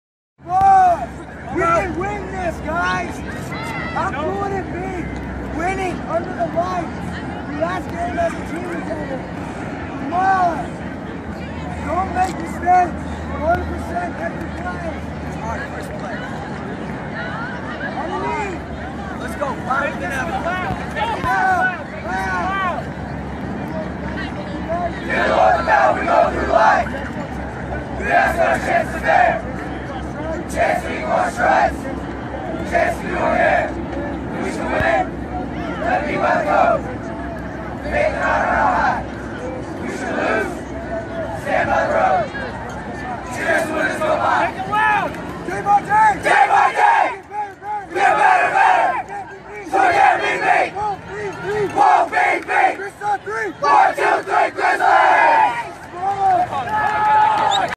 MRMS Grizzlies 8th Grade Pre Game Chant - October 7, 2014
MRMS Grizzlies 8th Grade Pre Game Chant for October 7, 2014 game against DCC Thunder. Last game of the Middle School Season, got to play under the lights at District 20 Stadium. Phenomenal group of kids, parents, and unbelievable coaches. These boys go out fired up and finish off DCC with a score of 38-0 in favor of the Grizzlies.